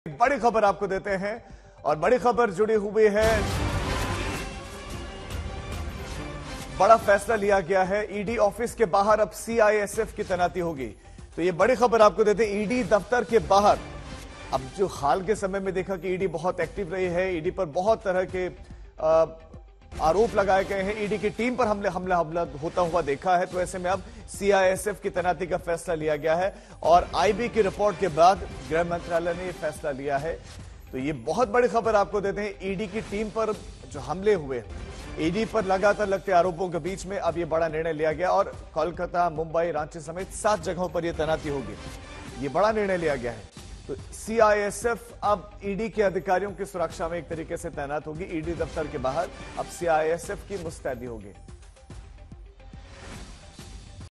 Breaking News: ED दफ्तरों के बाहर तैनात होंगे CISF जवान, गृह मंत्रालय का बड़ा फैसला
ईडी दफ्तर बाहर अब सीआईएसएफ की तैनाती रहेगी. आईबी की रिपोर्ट के बाद गृह मंत्रालय ने ये फैसला किया है.